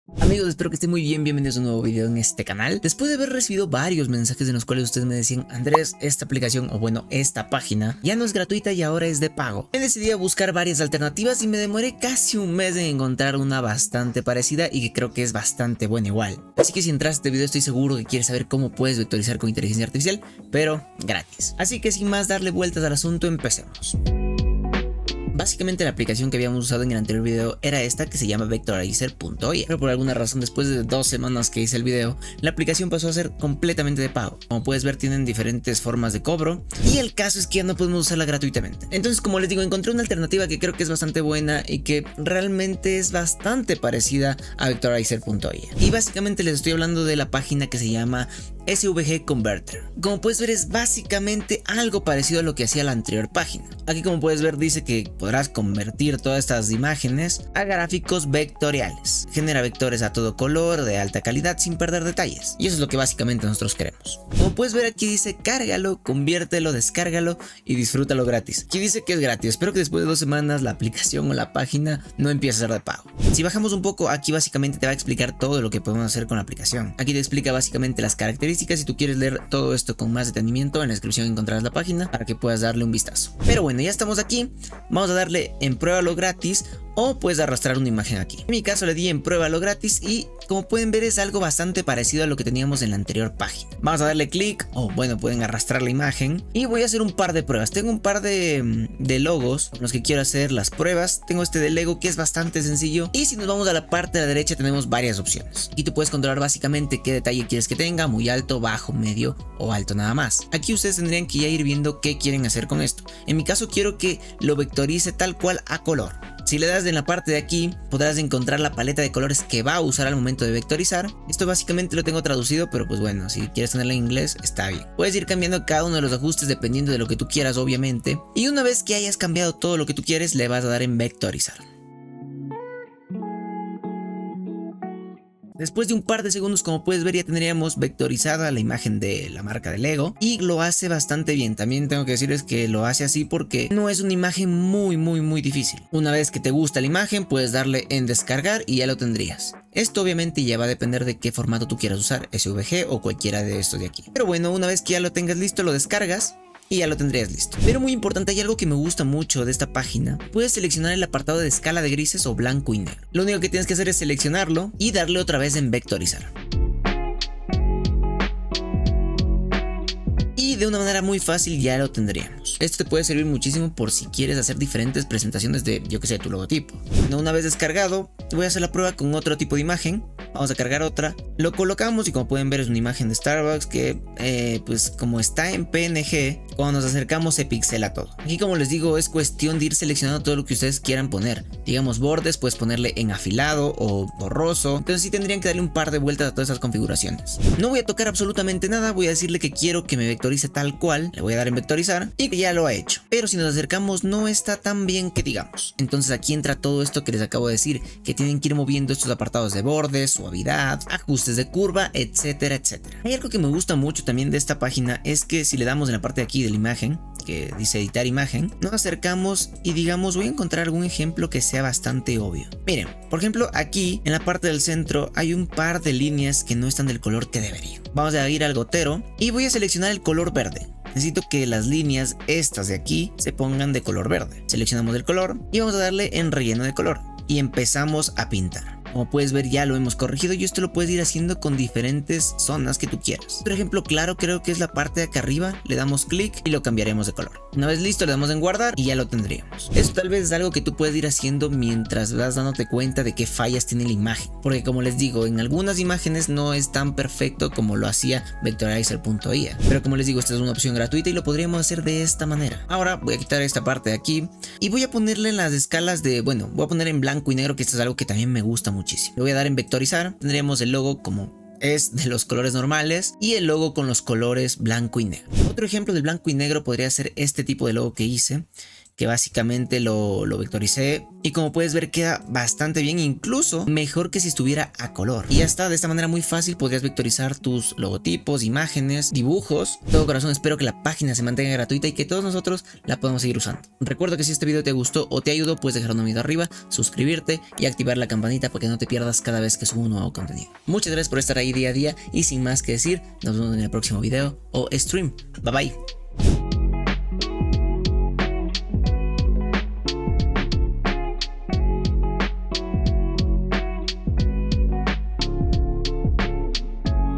🤯 VECTORIZAR IMAGENES CON INTELIGENCIA ARTIFICIAL GRATIS (ALTERNATIVA A VECTORIZER. IA )
En este video te voy a mostrar como puedes vectorizar imágenes con inteligencia artificial gratis
Esta es una alternativa a Vectorizer. Ia

🔥Compra los mejores complementos para crear el mejor contenido